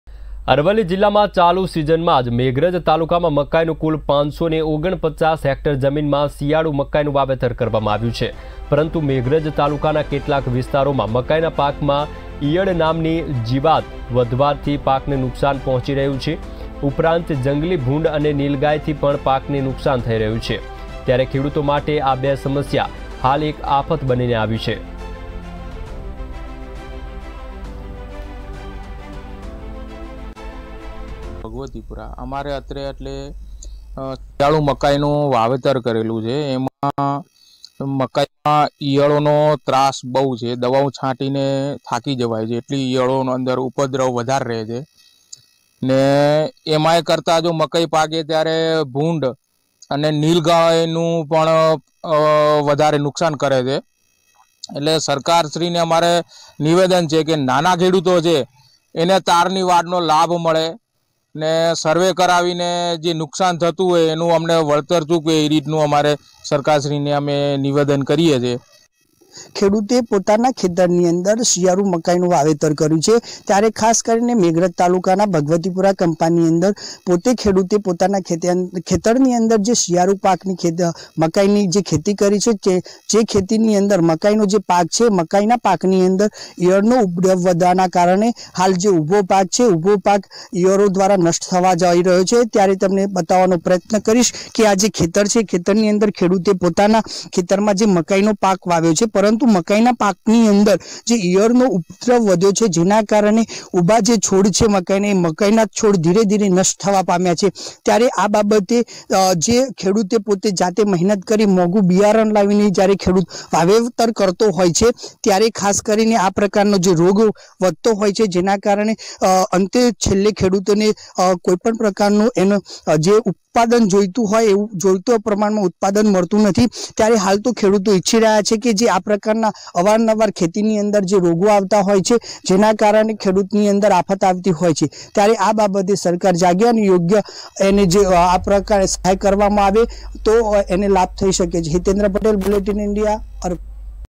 Maize plantation on 549 hectares of land in Aravalli | અરવલ્લીમાં 549 હેકટર જમીનમાં મકાઈનું વાવેતર
ARVALLI: મકાઈમાં ઈયર નામની જીવાત પડતા ખેડુતો મુશ્કેલીમાં
જંગલી ભૂંડ અને નીલ ગાયનો ત્રાસ પણ વધ્યો